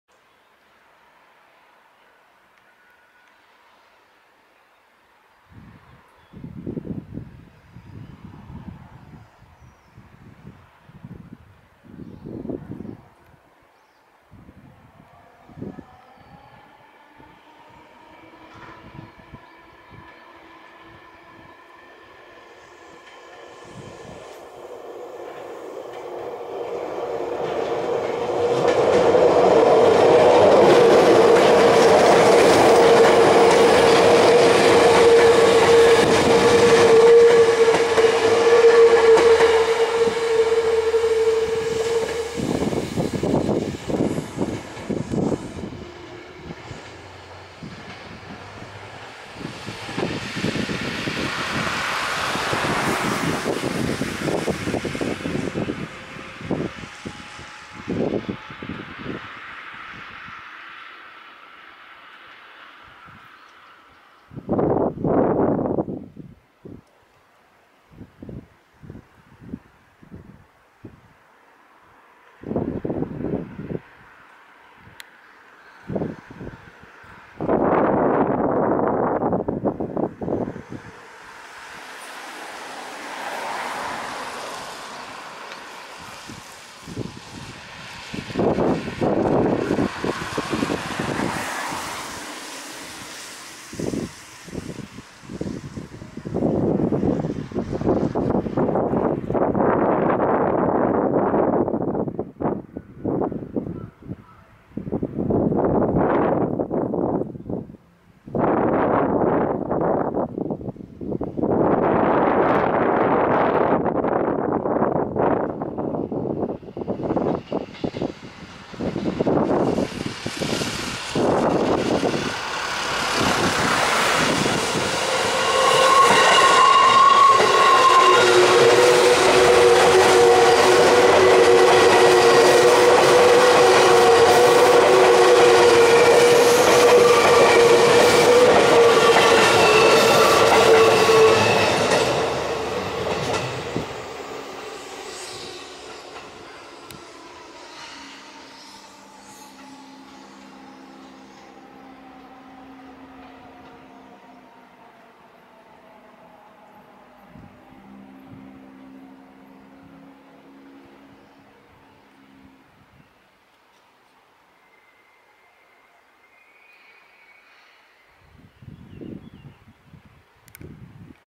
西武新101系263F（全M車）と新2000系2057F／武蔵大和＠SONY DSC-WX10
2012年3月31日（土）16時54分、西武多摩湖線・武蔵大和から上っていく各停「国分寺」行きの新101系263F（4両・全車モーター車編成）と、新宿線・小平から下ってきた各停「西武遊園地」行きの新2000系2057F（8両）です。2057Fは、折り返し急行「西武新宿」行きとなって新宿線に帰っていきます。